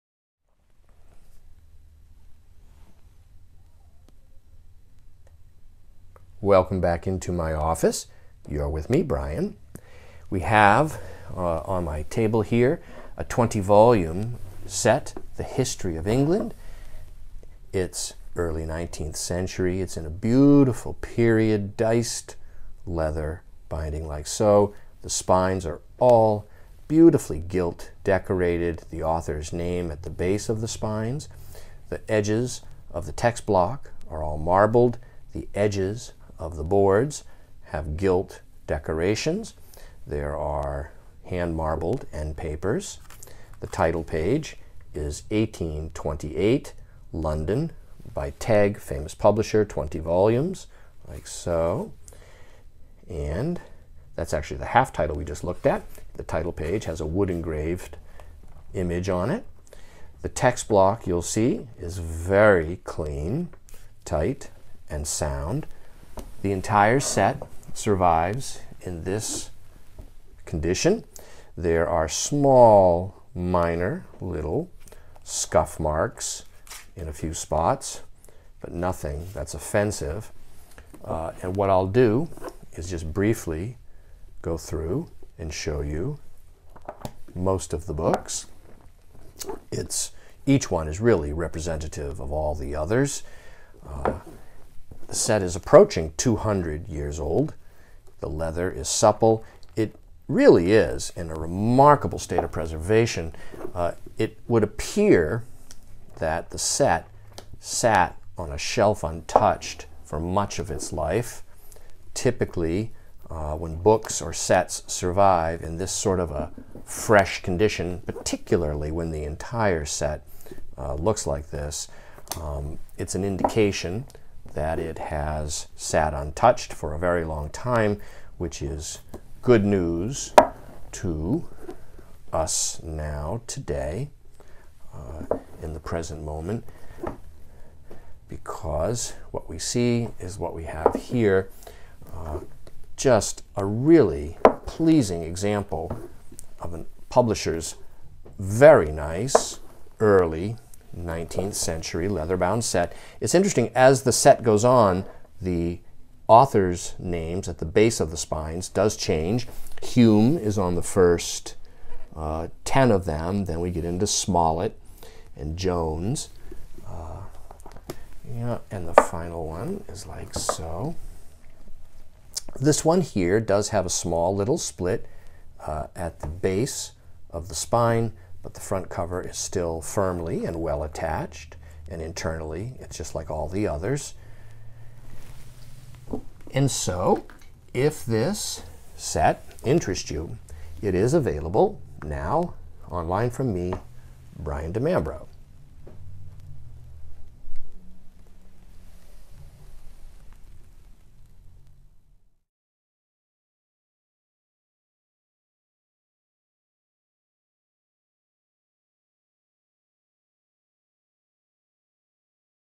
History of England Hume Smollett Jones 1828 lovely 20 vol. set | Brian DiMambro Virtual Book Tour
(Old Books, 19th Century Books, Leather Books, Decorative Books Bindings, British History, David Hume).

Short video captures condition much better than photos. Please watch prior to purchase to ensure a full understanding of the set.

The History of England from the Invasion of Julius Caesar to the Death of George the Third, by David Hume, Smollet and William Jones.
Published 1828, London for Thomas Tegg.
20 volumes, each c. 275 pages. Text in English. Engraved half titles.

Period full grained and diced leather bindings, raised gilt bands, title lettering and double ruled covers, board edges gilt patterned, marbled edges and end papers.

Set is approaching 200 years old and shows some signs of handling and time. Set is not new or in "new" condition.

Bindings gently age worn, leather lightly rubbed but still supple, joints slightly scuffed, final volume with some minor splitting along base of front joint, remains overall clean and attractive as an object.

Text block paper is gently toned, internally each volume remains clean, well preserved, tight, sound.

Still attractive on the shelf. Set displays a pleasing and unique age patina and is in nice antiquarian condition, any age flaws easy to overlook or forgive.

Please review photos for more detail and our best attempt to convey how this scarce complete antique set survives.

Book Measures c. 6 1/2" H x 4 1/4" W.

[B3582].